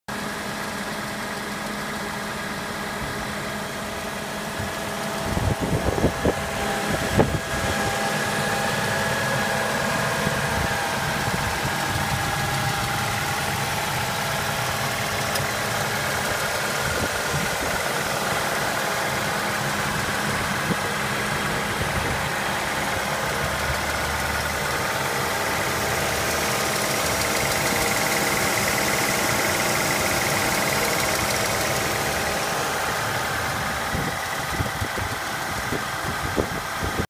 Auction #1577399 - 2009 Ford Escape Hybrid (16-1219 FO) SF
2009 Ford Escape Hybrid (16-1219 FO) SF (1W). Auction #1577399 being auctioned by State of Washington Surplus Operations